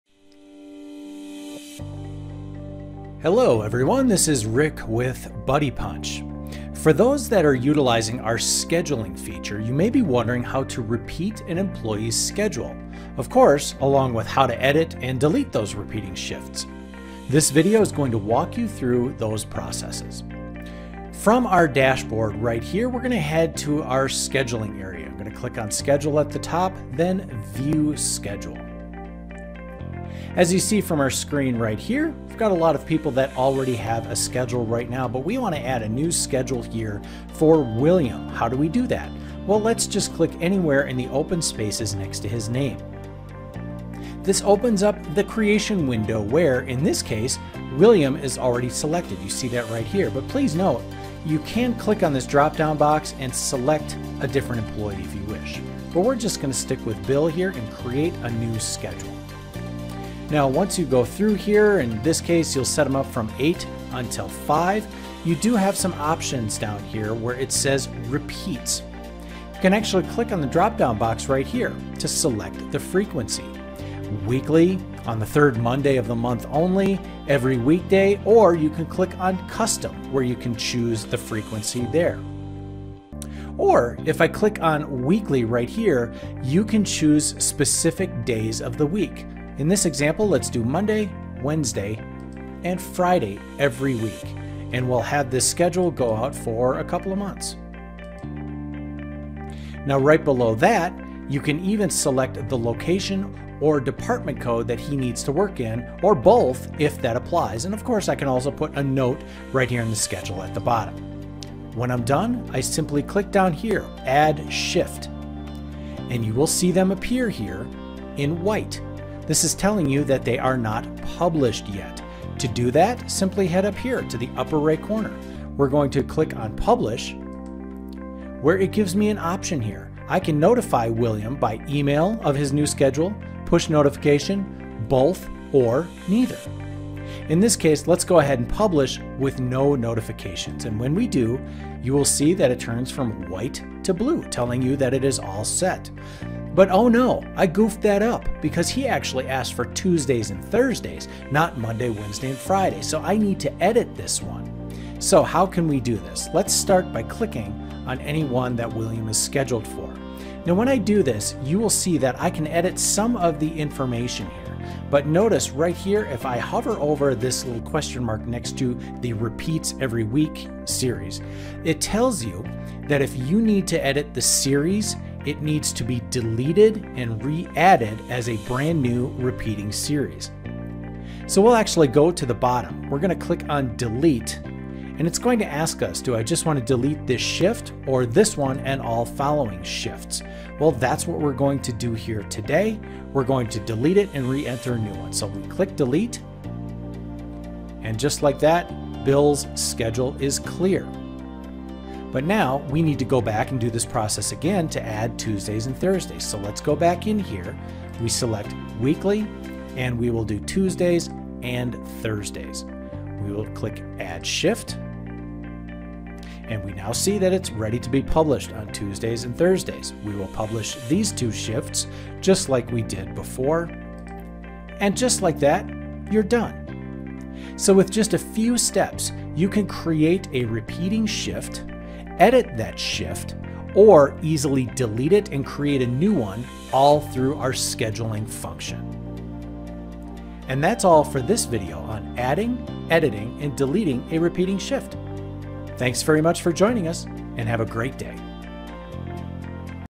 Adding and Editing Repeating Shifts in Buddy Punch
When using Scheduling with Buddy Punch you can create shifts that repeat.  This video shows you how to create and update them.